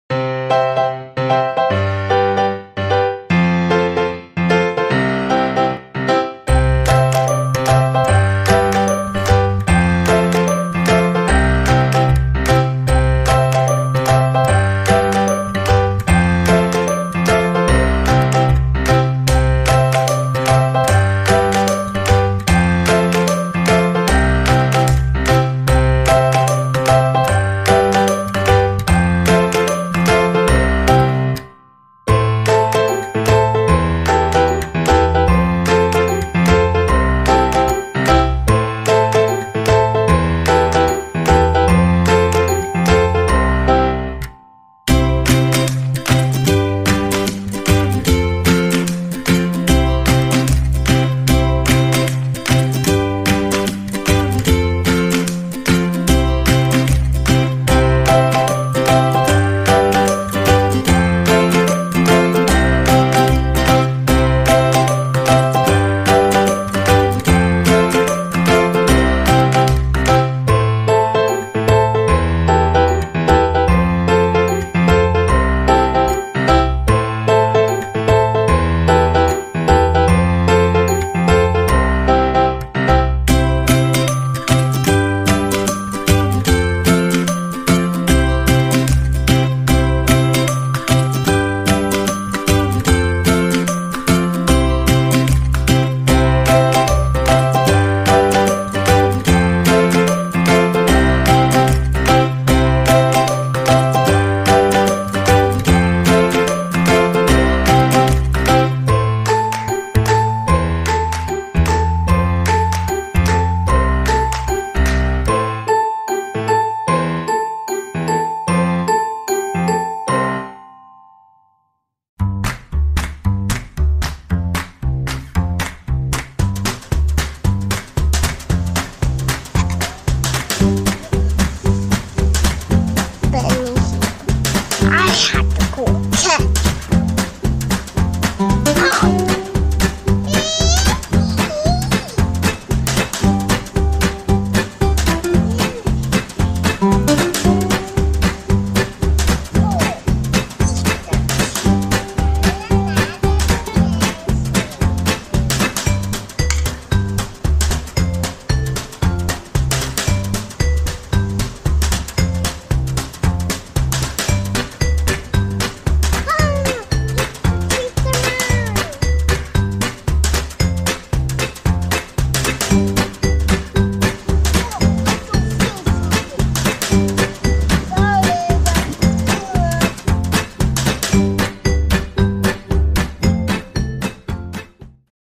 Catch the Easter Bunny
Late Upload. Recorded On Easter 2019! Watch and see how JUJU catches the Easter Bunny, and get's ALL the Easter Treats, but pays for it later! Cute and funny. One of JUJU's first projects.